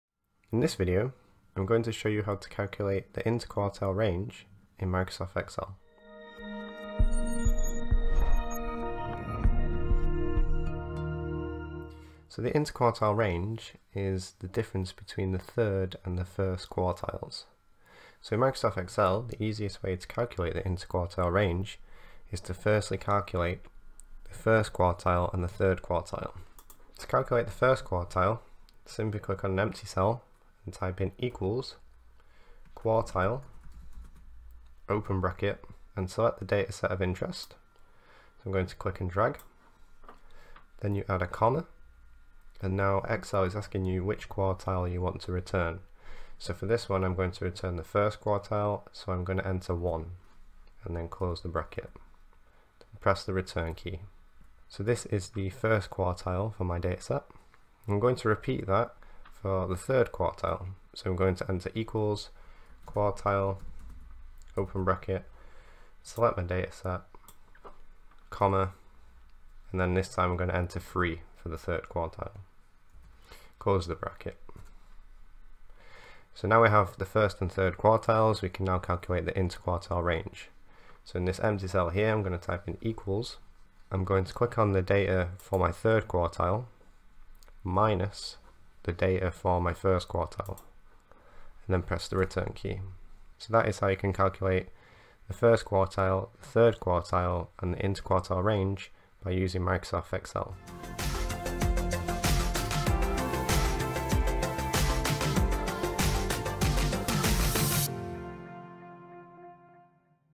How To Calculate The Interquartile Range In Excel
In this video tutorial, I will show you how to calculate the first (Q1) and third (Q3) quartiles of a dataset, and how to use these to create the interquartile range (IQR).

Excel version used: 365 ProPlus